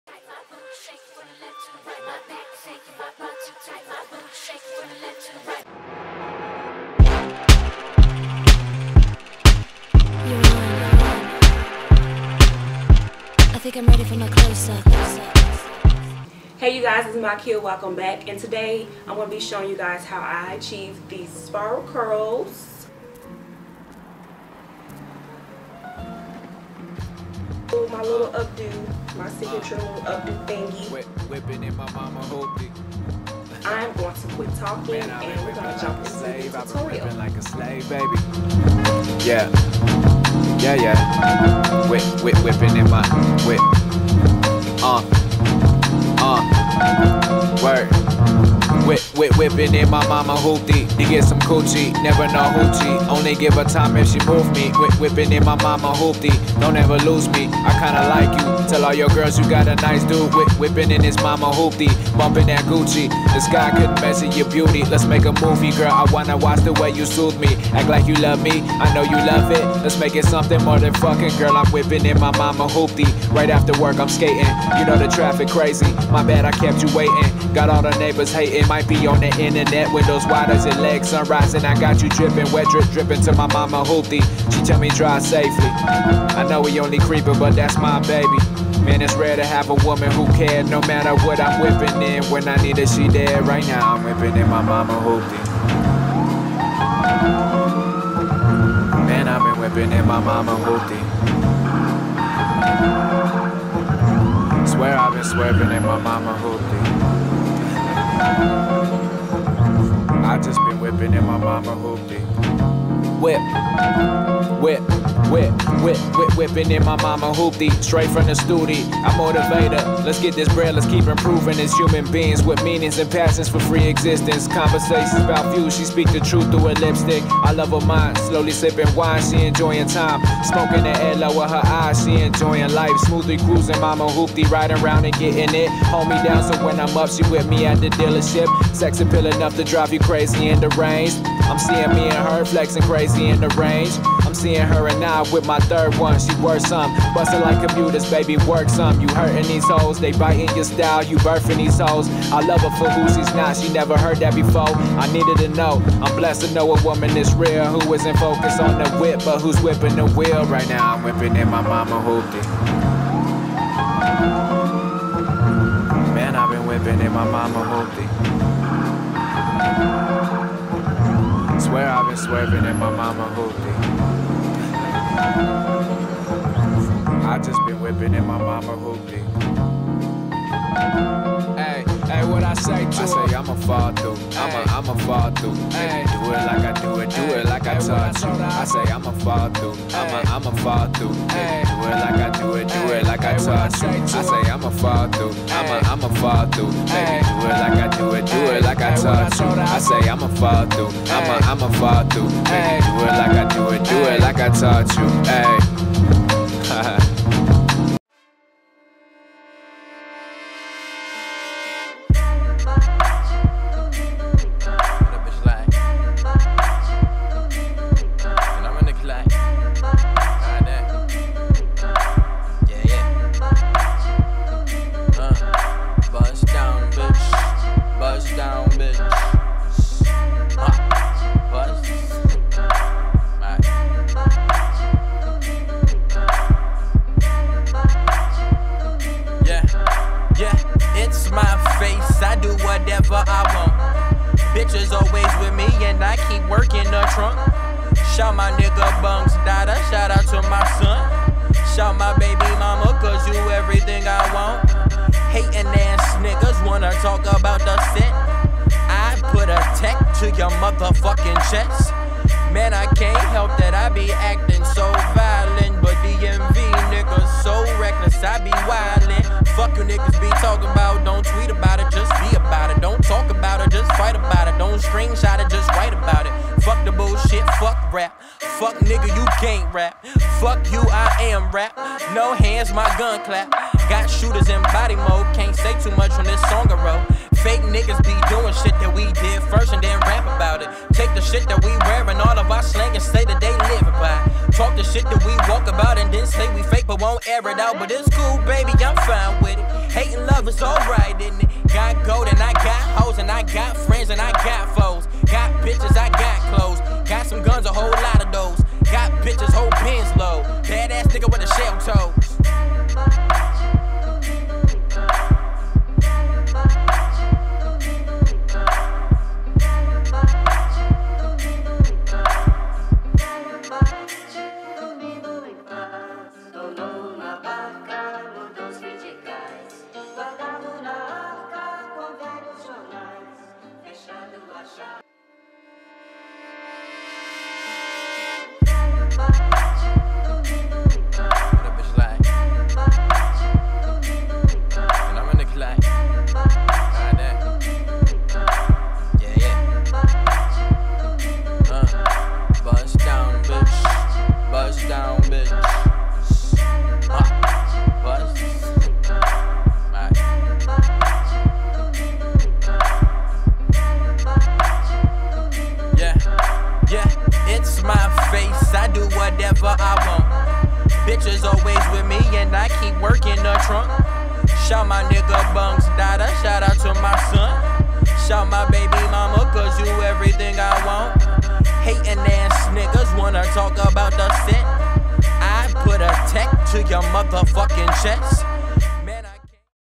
PERFECT PERM ROD SET TUTORIAL for Natural Hair ! | MikeyaOnly
Hey loves. I have been experiencing on my hair lately, and i just been loving how my hair has been coming out. If you guys like natural hair videos show me some love. We gone keep videos cranking out!

Products:
Taliah Waajid Curl Sealer
Shea Butter mix
Vigorol Moouse
Dr. Miracles Nape and Temple

Tools:
Perm rods
deman brush
Hair dryer